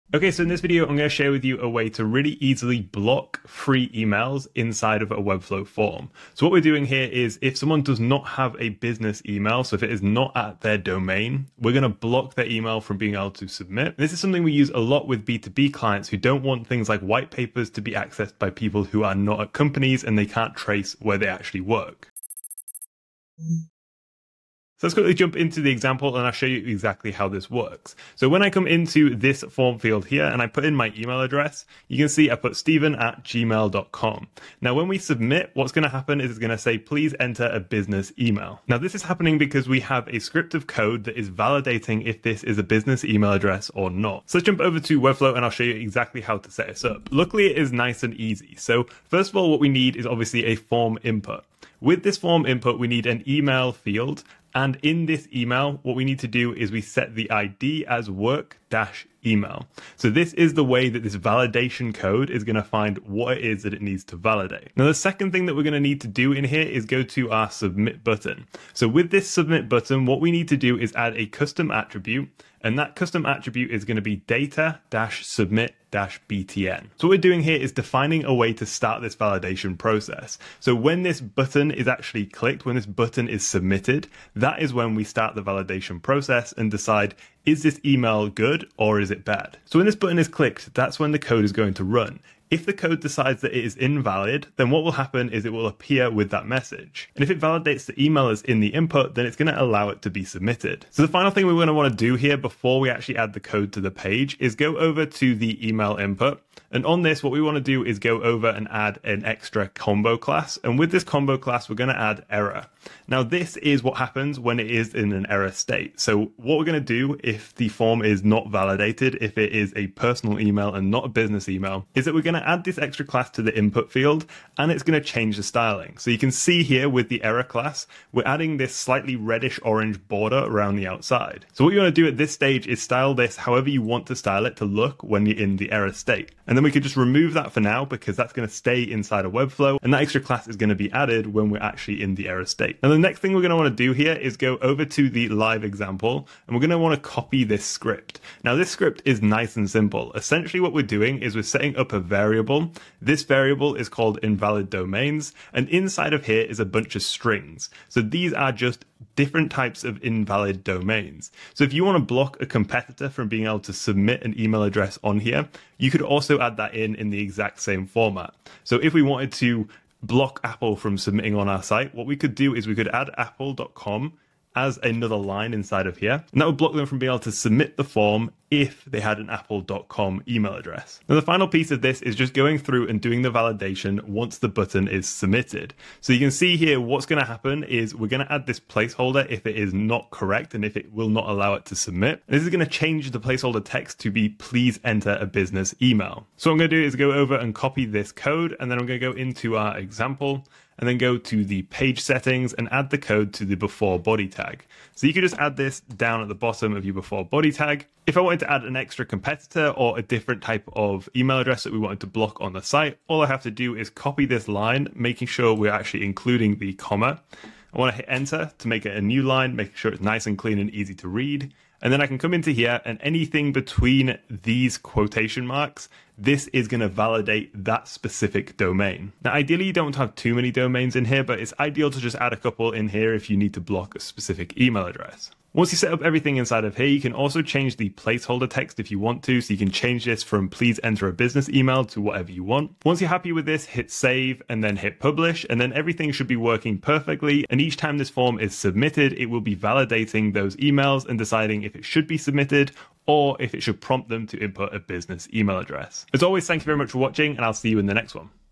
How to Block Personal Emails in Webflow Forms
In this video I share how we block personal (non-business) emails in Webflow forms.